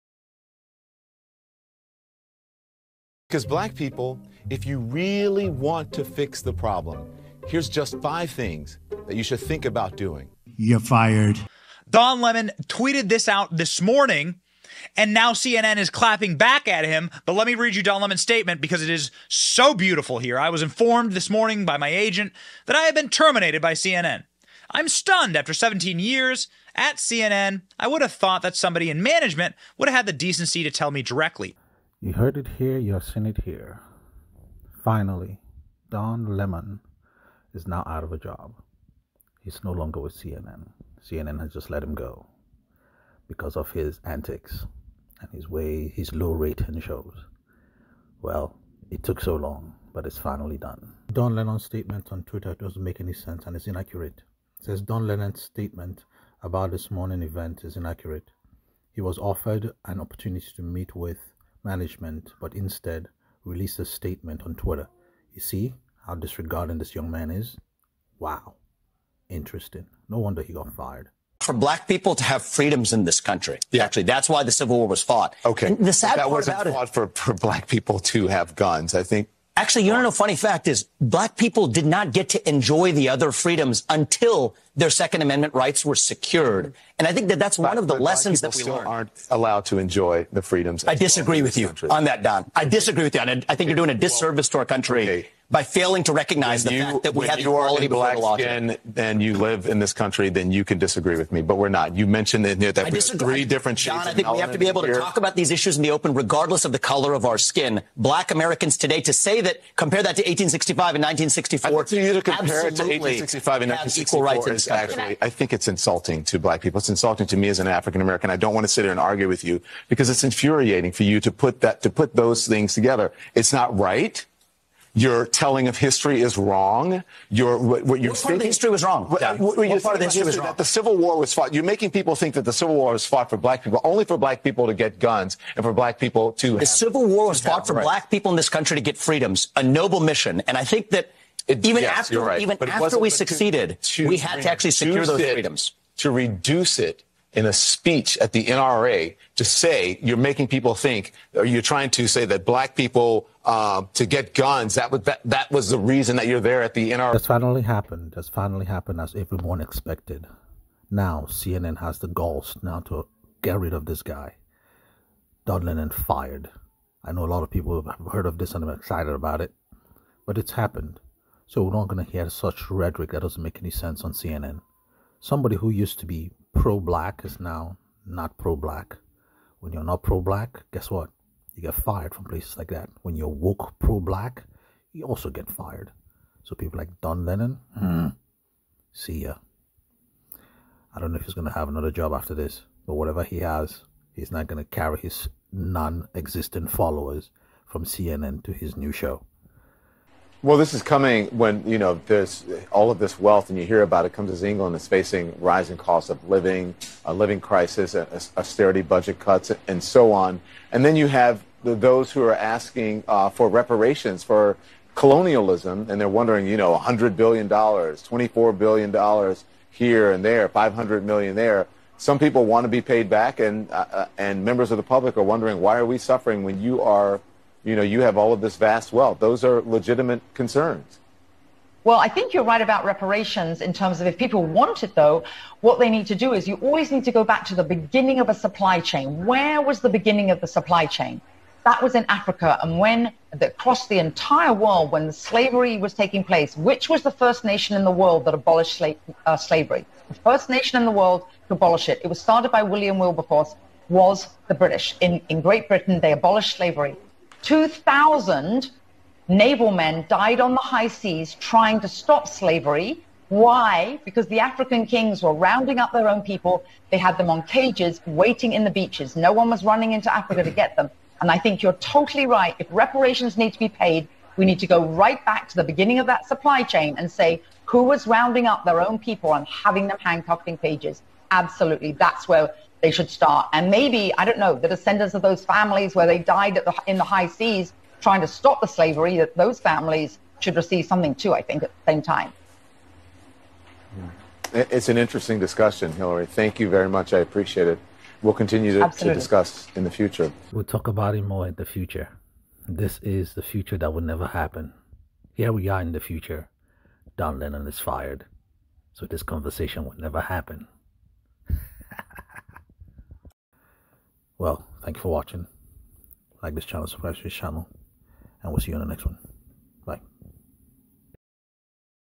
CNN FIRES Don Lemon | Total Historical Meltdown
We were right, CNN has been planning to fire Don Lemon this whole time